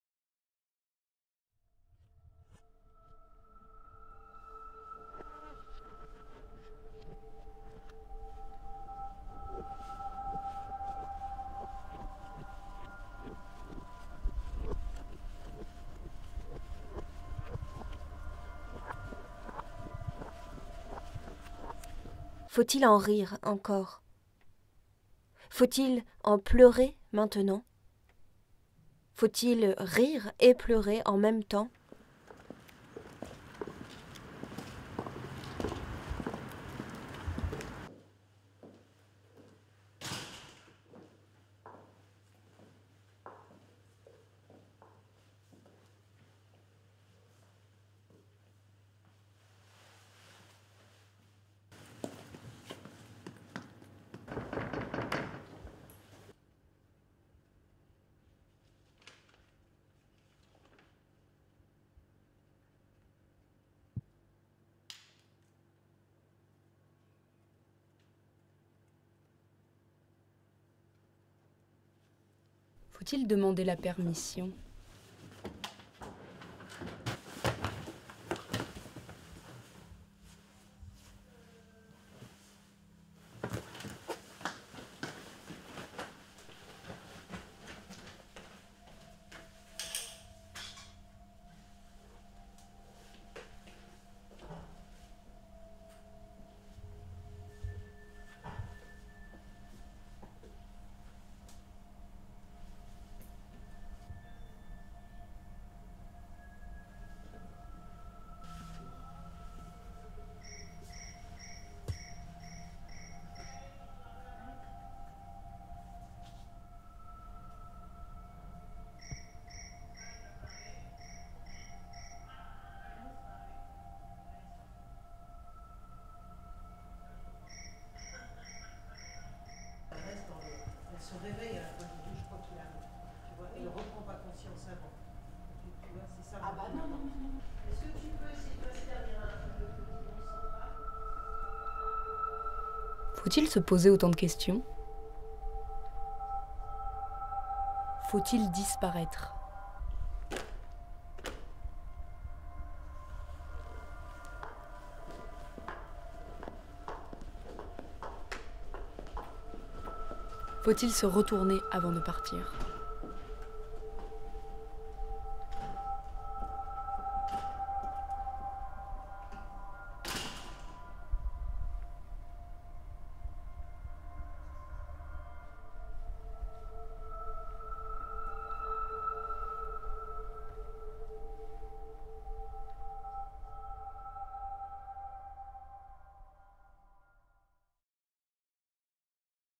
FAUT-IL #3
Errance poétique aux Îlets pour ce troisième opus en théâtre confiné.

FAUT-IL : une mini série en 8 épisodes à découvrir en ligne jusqu'au 18 novembre pour une balade quotidienne dans les coulisses de la création à huis clos de FAUT-IL LAISSER LES VIEUX PÈRES MANGER SEULS AUX COMPTOIRS DES BARS (texte & mise en scène Carole Thibaut).

réalisation Hugo Anguenot, Chloé Bouiller, Léo Nèti & Louise Héritier • montage Léo Nèti • avec la participation de toute l'équipe du théâtre des Îlets

production théâtre des Îlets – CDN de Montluçon – région Auvergne Rhône-Alpes • novembre 2020